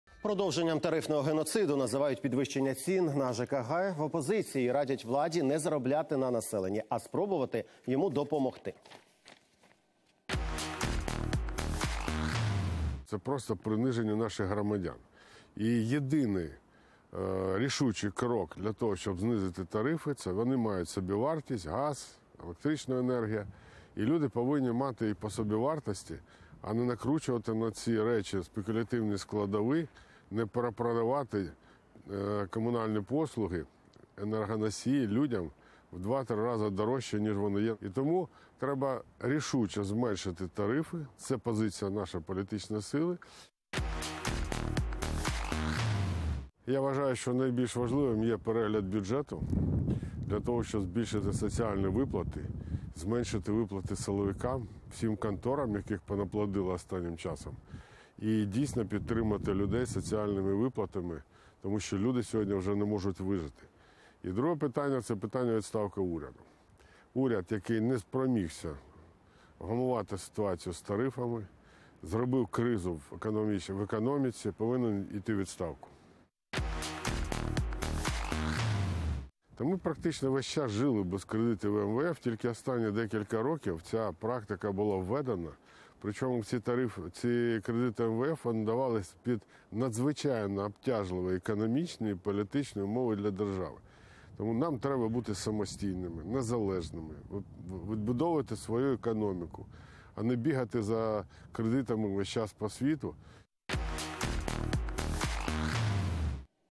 "Опозиційна платформа - За життя" закликає зупинити тарифний геноцид в Україні - Юрій Бойко
Продовженням тарифного геноциду називають підвищення цін на ЖКГ в опозиції - і радять владі не заробляти на населенні, а спробувати йому допомогти